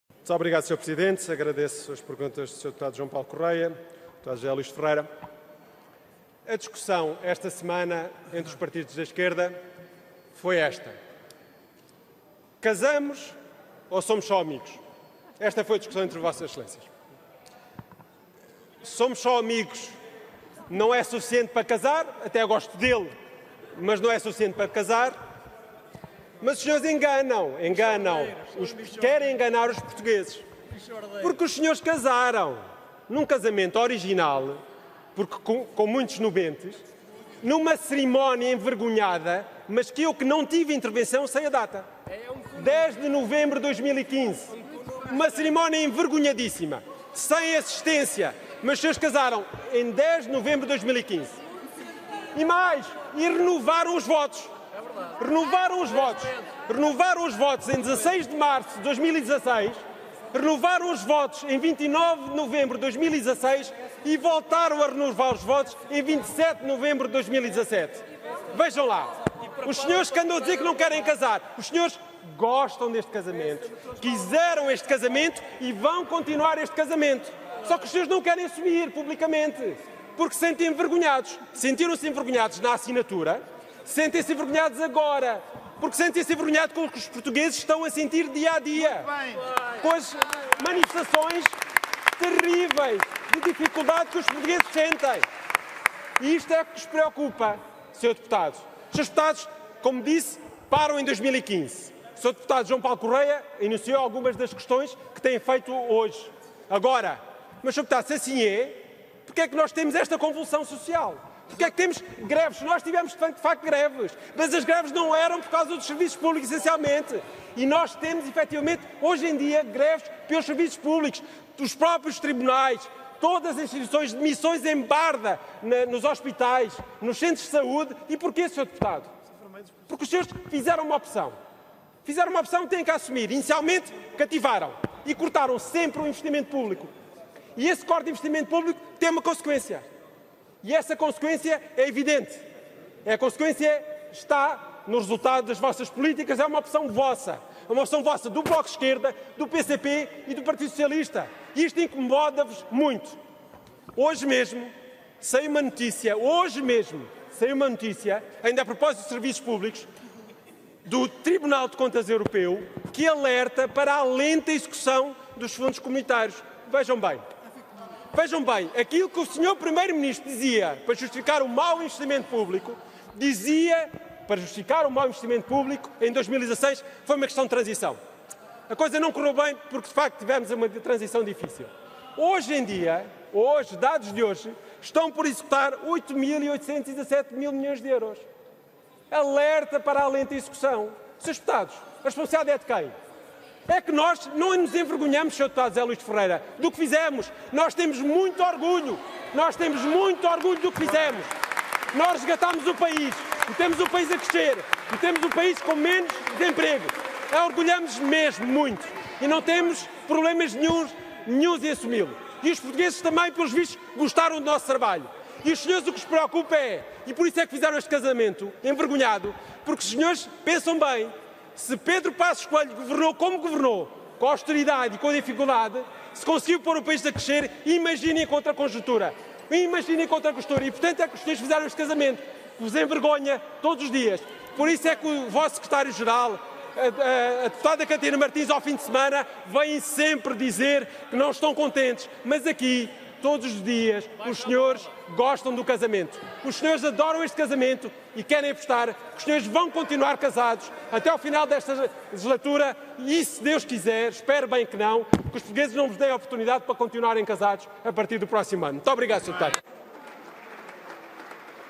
Pedro do Ó Ramos responde a deputados do PEV e PS
Intervenção do social-democrata no debate, agendado pelo PSD, sobre o tema "a qualidade da resposta dos serviços públicos".